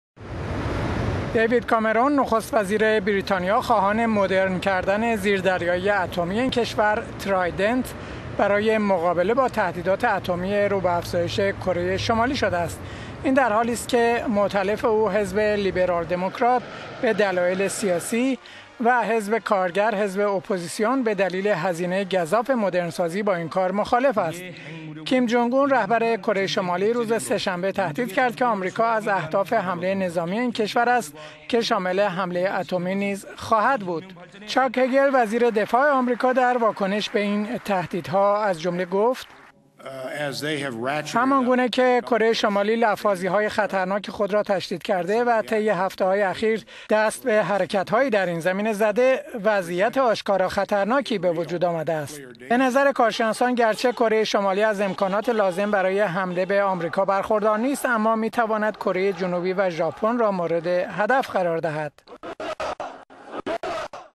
نخست وزیر بریتانیا خواهان مدرن سازی زیر دریایی اتمی...
دیوید کامرون نخست وزیر بریتانیا، خواهان مدرن کردن زیردریایی اتمی این کشور، ترایدنت، برای مقابله با تهدیدات اتمی رو به افزایش کره شمالی شده است. این در حالی است که موئلف او حزب لیبرال دمکرات، به دلایل سیاسی و حزب کارگر، حزب اپوزیسیون، به دلیل هزینه گزاف مدرن سازی، با این کار مخالف است.

کیم جونگ اون رهبر کره شمالی، روز سه شنبه تهدید کرد که آمریکا از اهداف حمله نظامی این کشور است که شامل حمله اتمی نیز خواهد بود.

چاک هگل، وزیر دفاع آمریکا در واکنش به این تهدید ها گفت:
"همانگونه که کره شمالی لفاظی های خطرناک خود را تشدید کرده و طی هفته های اخیر دست به حرکت هایی در این زمینه زده، وضعیت  آشکارا خطرناکی به وجود آمده است."

به نظر کارشناسان، گرچه کره شمالی از امکانات لازم برای حمله نظامی به آمریکا برخوردار نیست، اما می تواند با موشک های میان برد خود اهداف نظامی هم پیمانان آمریکا در کره جنوبی و ژاپن را مورد هدف قرار دهد.